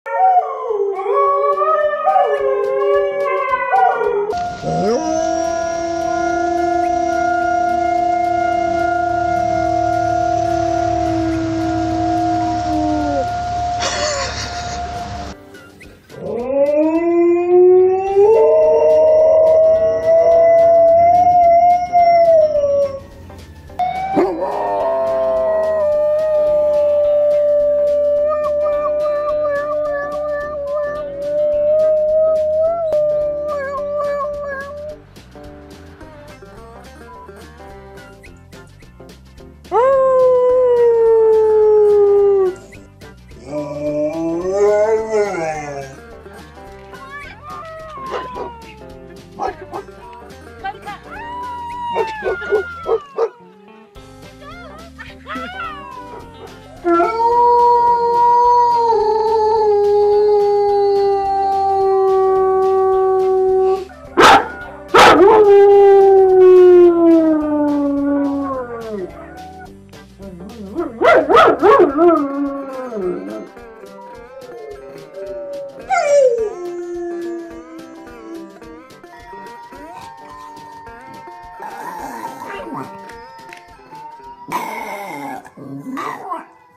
Funniest dog howling compilation 2016
Best compilation of howling dog 2016, enjoy ! :D
Relax video, very enjoyable :)